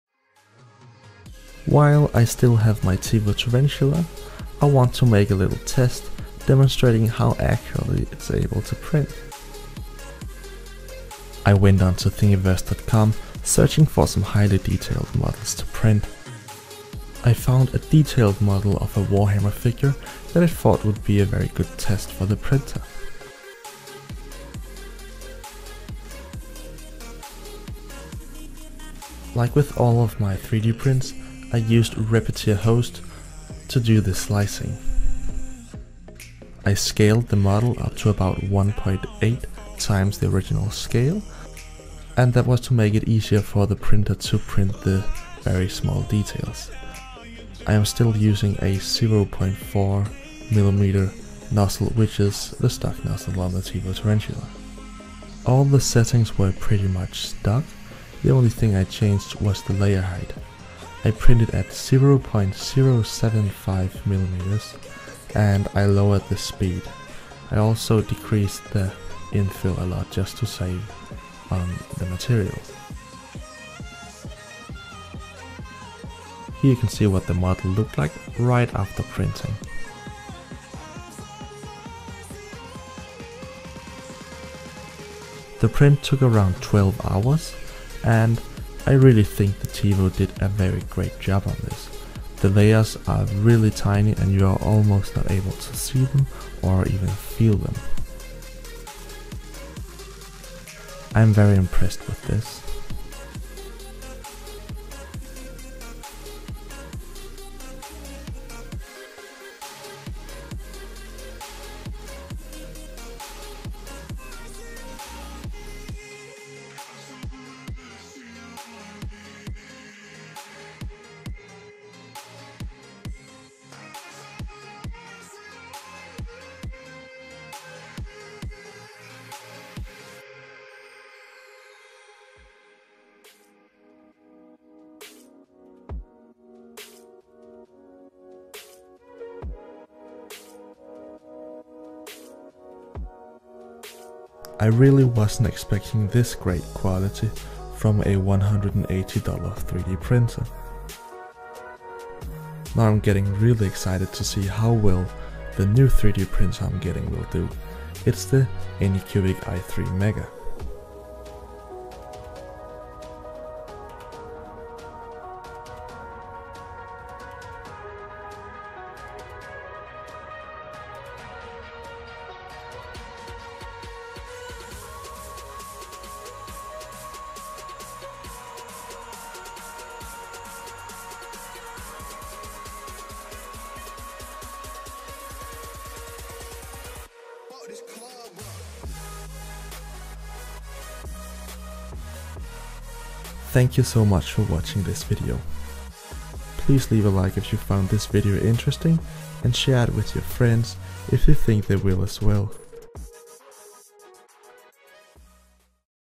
Highly detailed 3D prints with Tevo Tarantula
In this video will I be demonstrating the Tevo Tarantula's printing capabilities. I am printing a detailed Warhammer 40k figure that you can download from this

I was very impressed with the fine details the printer was able to produce.

Note: This was printed in PLA